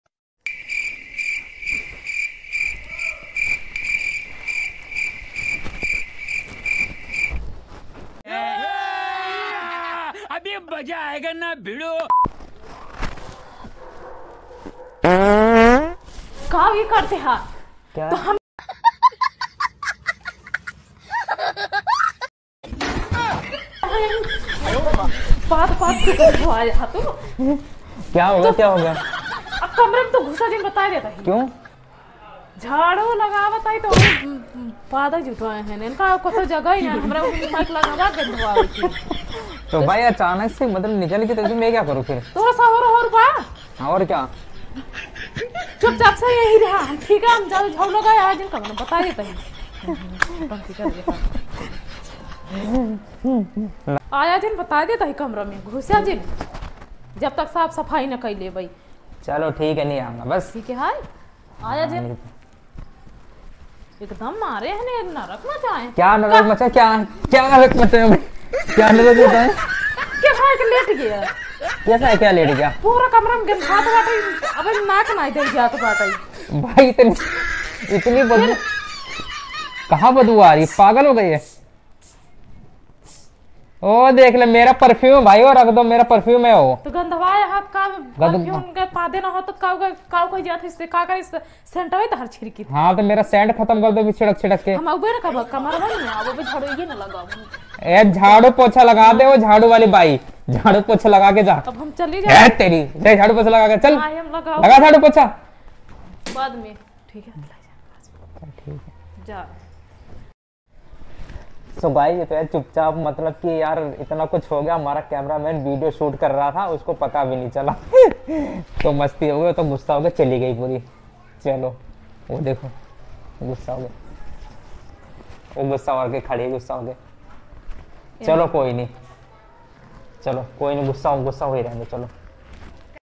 Apni Wife ke Muh per pad Mara
Aaj Bhai bhejte Ho Gai wife ke samne pad nikal